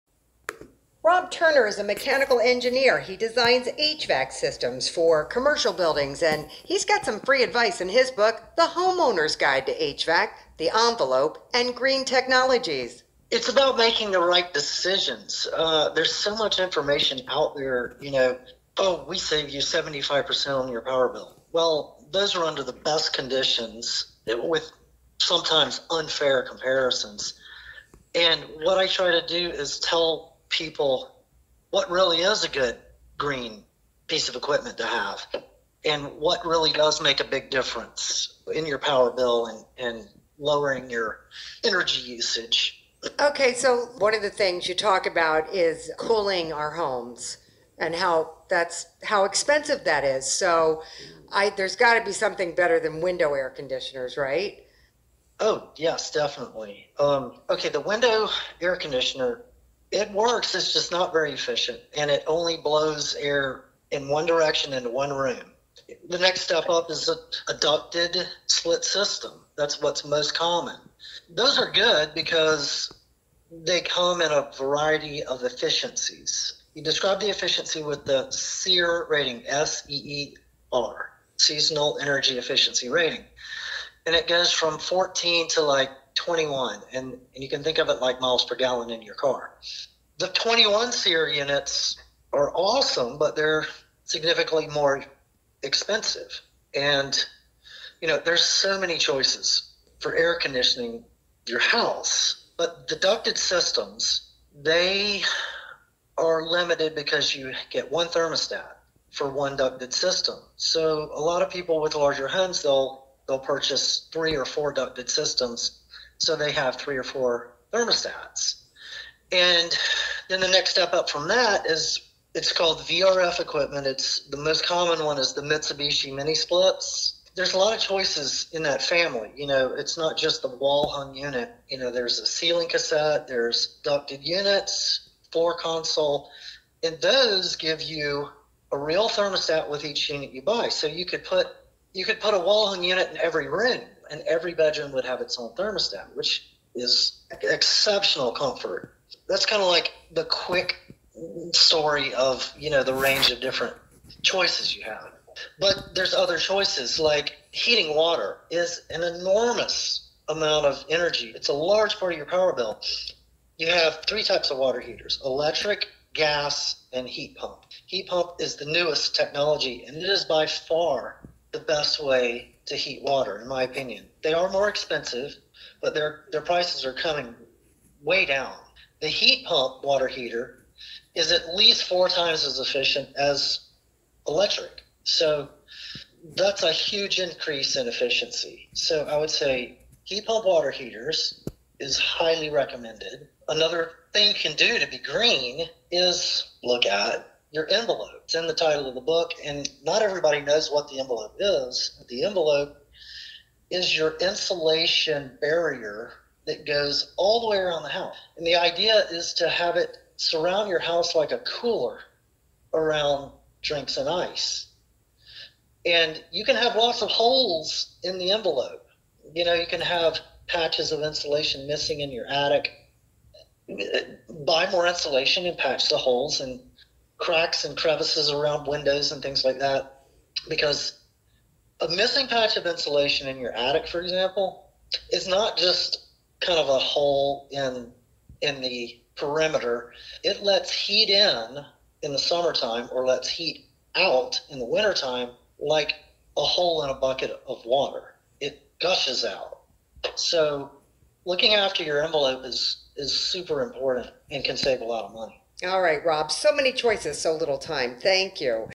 Rob's Podcast Radio Interview for his book: The Homeowner's Guide to HVAC
The book title in full is:  The Homeowner's Guide to HVAC, the Envelope, and Green Technologies.  I talk about how to save money in your house with your air conditioning (HVAC), your water heating, and your electrical.  The book was published by Page Publishing.  Just search Amazon for "homeowner hvac" and it is like the 5th book down.  I hope you get a copy and enjoy it.  The book is particularly good if buying a new house or renovating a house.  Lots of good honest advice.  Please don't forget to leave a book review if the book was helpful to you.